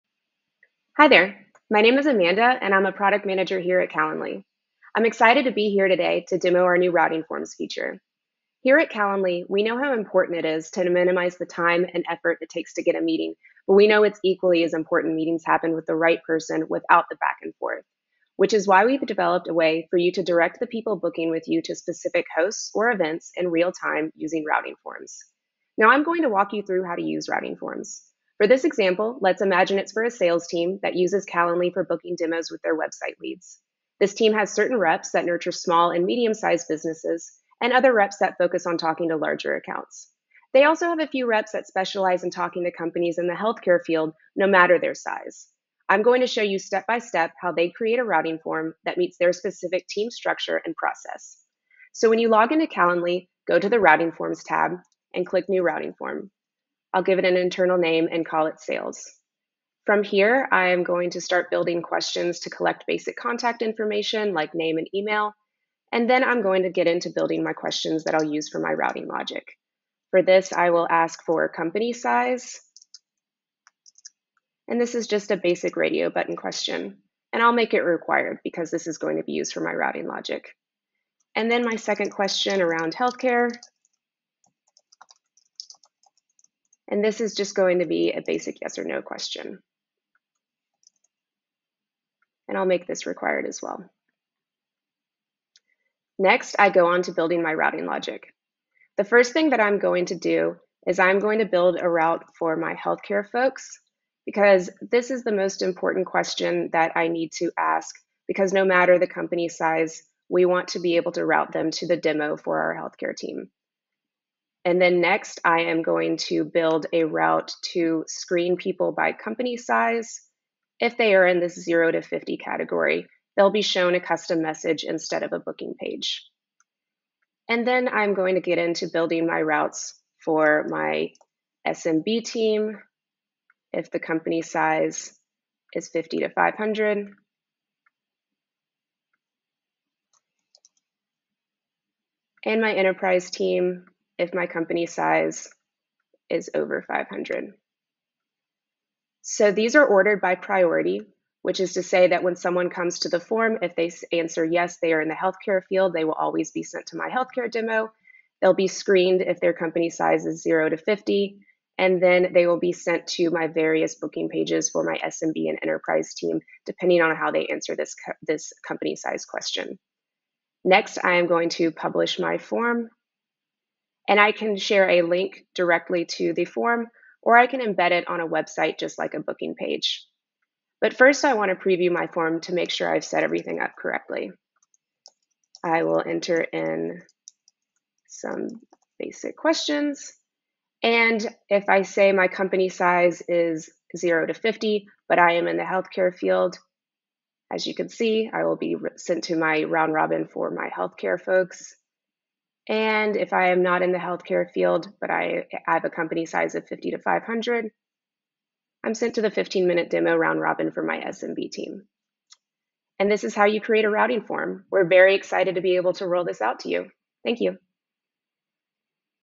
Calendly's Routing Forms Demo
Amanda Lynn, Product Manager at Calendly, will walk you through Calendly's routing forms feature in this quick demo.

She'll explain how to:
- create questions for your form
- set up routing logic in minutes
- preview your form
- share your form (via a link or an embed on your website)

We know how important it is to minimize the time and effort it takes to get a meeting, and we know it's equally as important that meetings happen with the right people, without all the back and forth.

In this demo, Amanda will show you step-by-step how a theoretical sales team might set up a routing form to cater to their team's structure and specific needs.